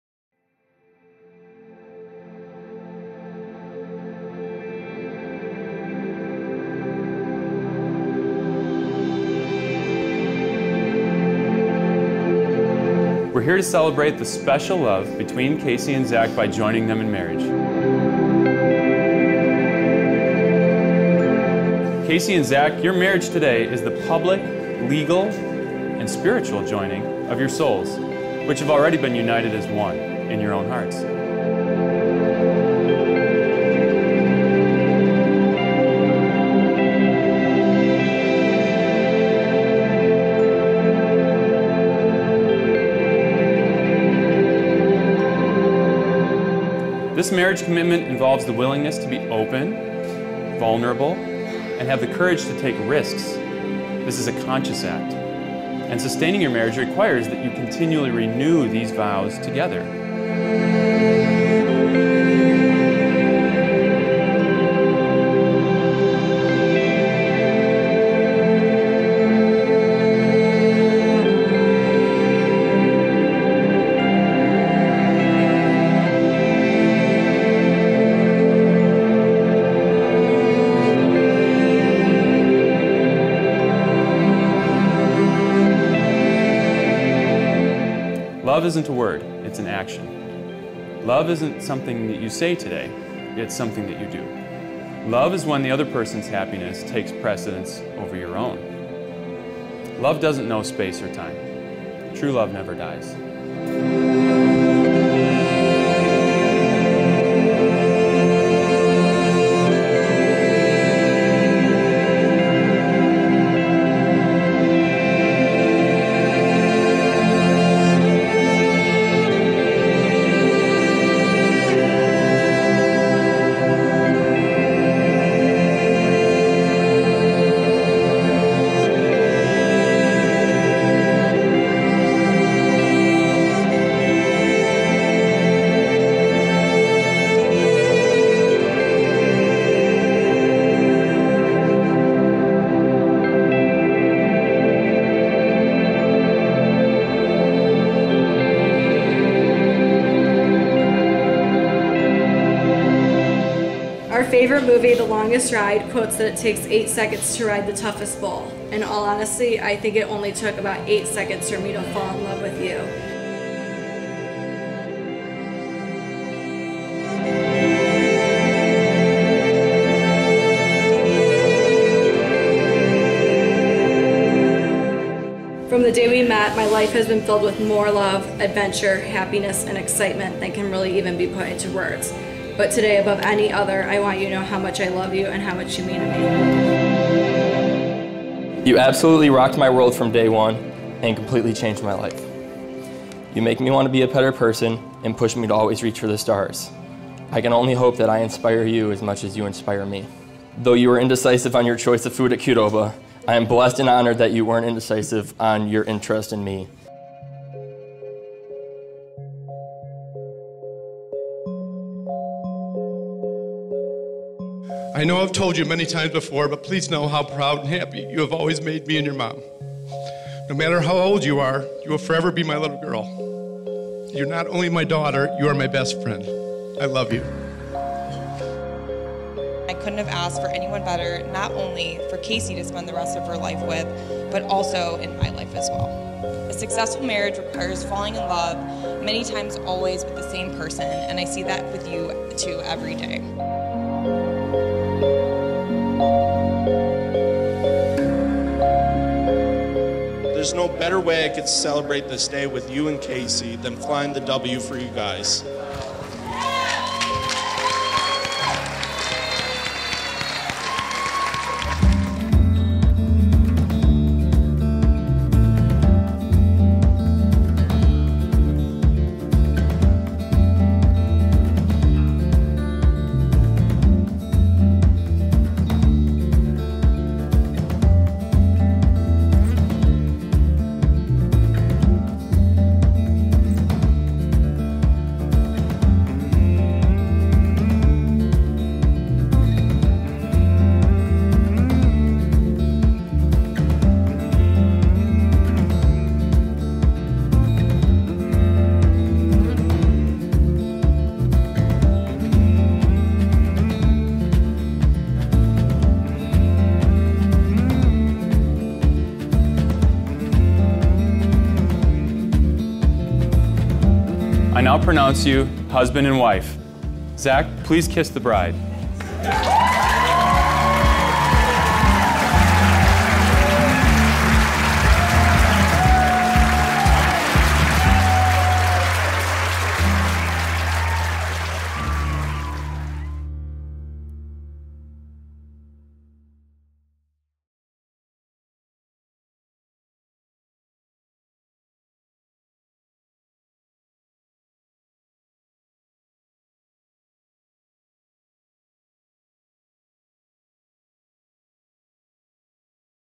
Mike Staff Productions - Plymouth Wedding Videography - The Wedding Video of Kacy and Zack
Filmed: May 24, 2019
Ceremony: McClumpha Park (Plymouth Township Park)
Romantics: Downtown Plymouth
Reception: Meeting House Grand Ballroom, Plymouth, Michigan